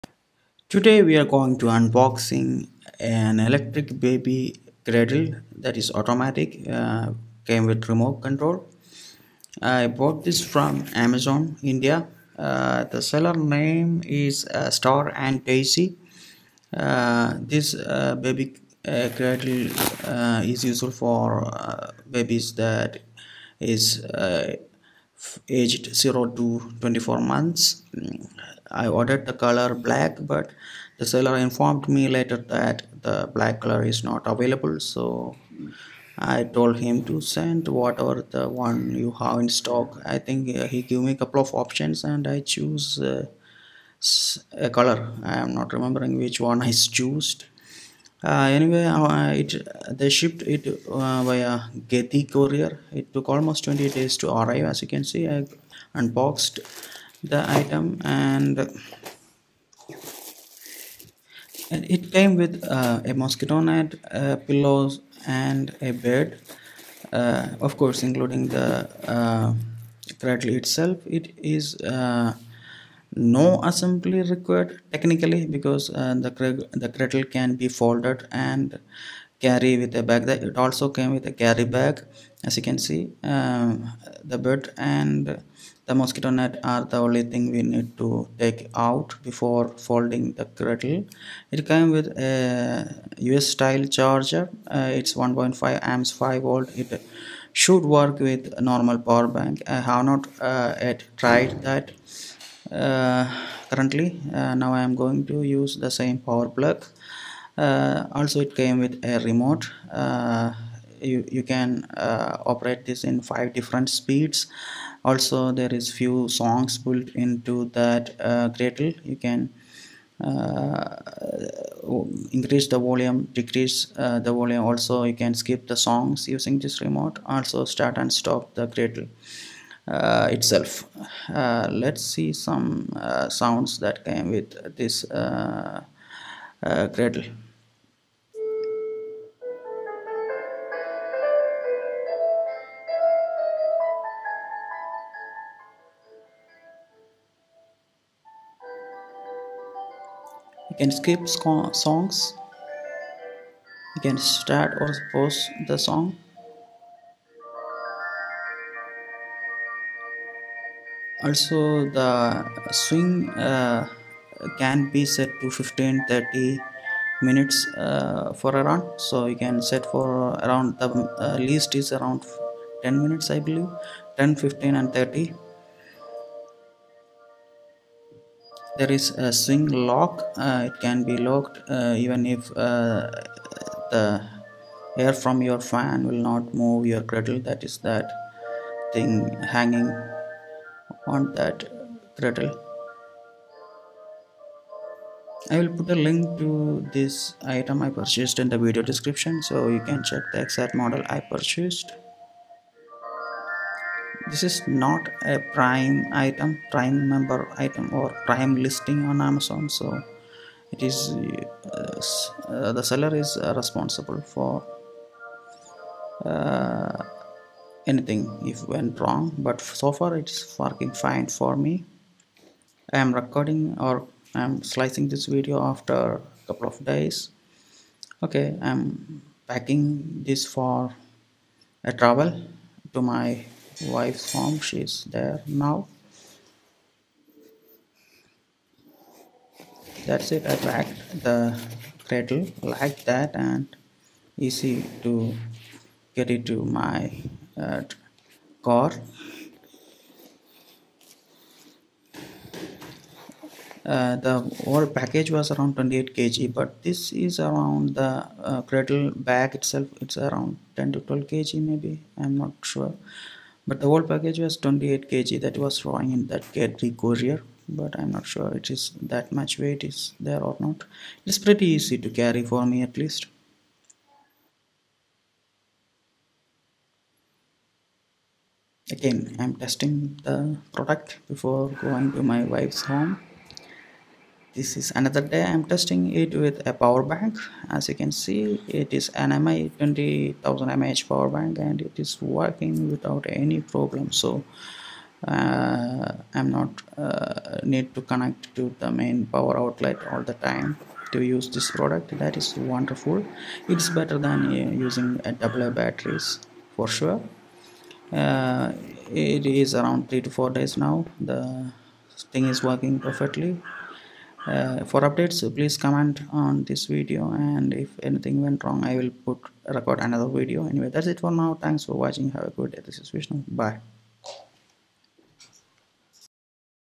Electric Baby Cradle with Automatic Swing & Remote Control by StarAndDaisy
Amazon India
Amazon USA
Electric Baby Cradle with Automatic Swing & Remote Control
StarAndDaisy Electric Baby Swing Cradle with Remote Control Rocking cot Newborn Sleeping Bed Crib Best Suited for 0-24 Months (Beige)
5 Gear Automatic Electric Swing: Say good bye to old fashioned hand-pushed cradle. It imitates your hand push more accurately with electricity and gives you the freedom to control the speed of the swing to soothe your baby and let him feel in your arms.
Wireless Remote Control: Have the freedom to keep doing your stuffs while ensuring your baby is at comfort all the time. If the swing speed is not fit or the music is too loud, adjust it at ease with the wireless remote control.
Swing Lock: The bed can be locked to the frame if the parent thinks the need that the swing is not required. It would then be used as a normal bed.
Bionic Mother: We know there is no replacement to mother’s care in this world but we try to make efforts to imitate this care and love using the technology.